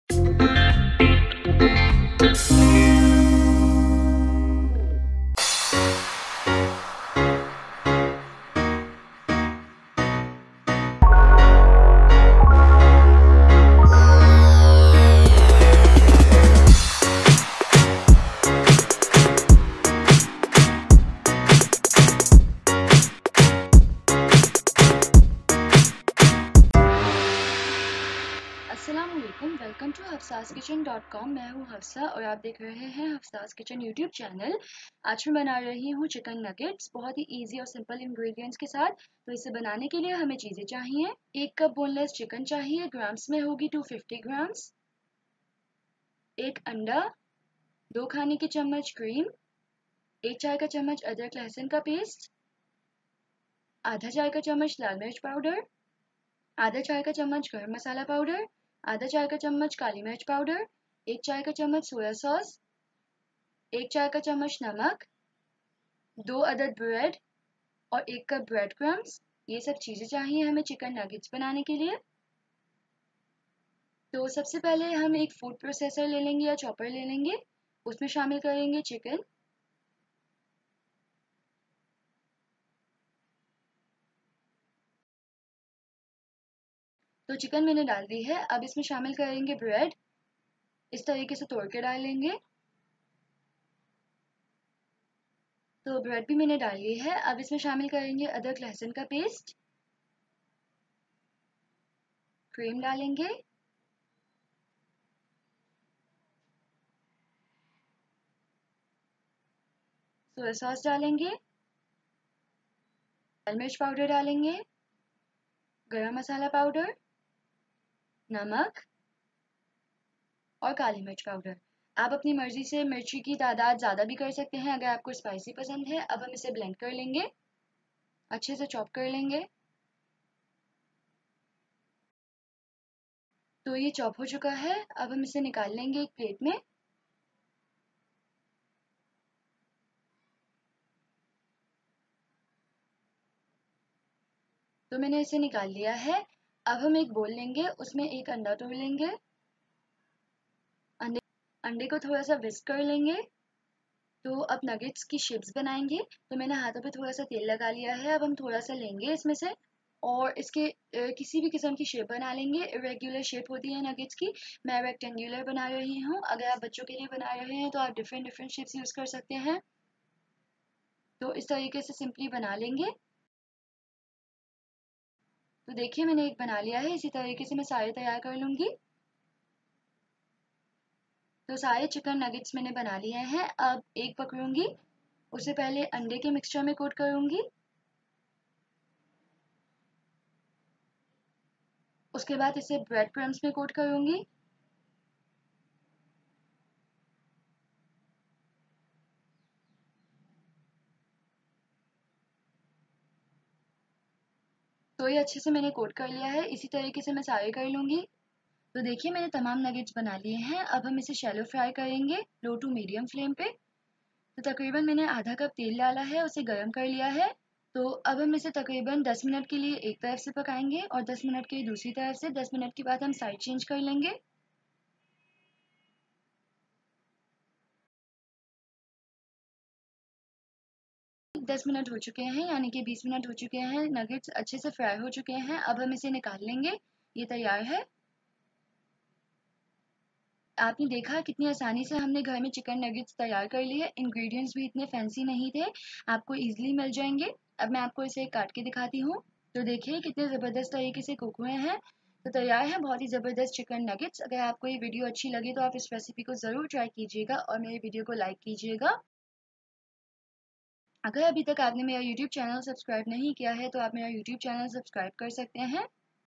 CHICKEN NUGGETS | چکن نگٹس -KIDS LUNCH BOX RECIPE - HOW TO MAKE CHICKEN NUGGETS AT HOME
HOW TO MAKE CHICKEN NUGGETS.A VERY EASY AND YUMMY RECIPE OF CHICKEN NUGGETS.A STEP BY STEP RECIPE OF CHICKEN NUGGETS BY HAFSA'S KITCHEN

INGREDIENTS:

1 CUP CHICKEN
1 EGG
2 TBSP CREAM
1 TSP GINGER GARLIC TASTE
1/2 TSP RED CHILLI POWDER
1/2 TSP GARAM MASLA POWDER
1/2 TSP BLACK PEPPER POWDER
1 TSP SOYA SAUCE
1 TSP SALT
2 BREAD SLICES
1 CUP BREAD CRUMBS